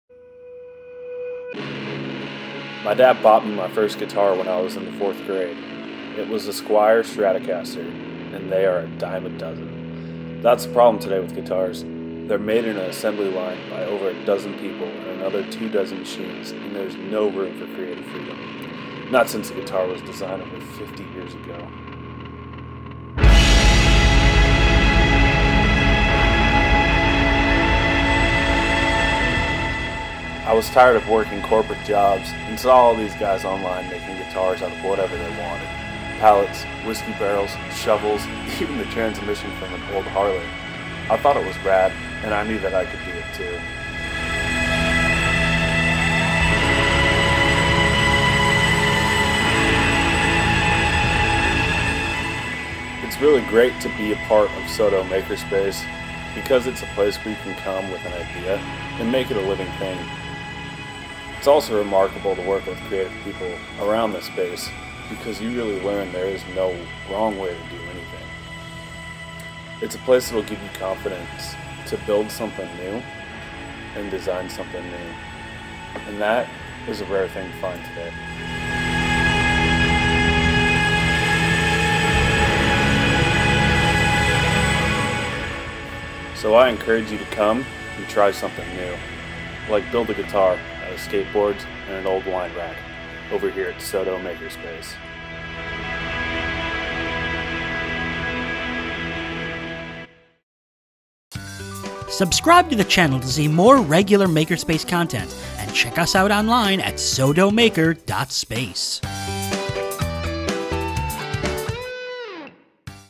Making Guitars at SoDo MakerSpace!
Why buy a guitar when you can make your own? Drew Mycroft talks about his experience at the SoDo MakerSpace, making his own custom guitar.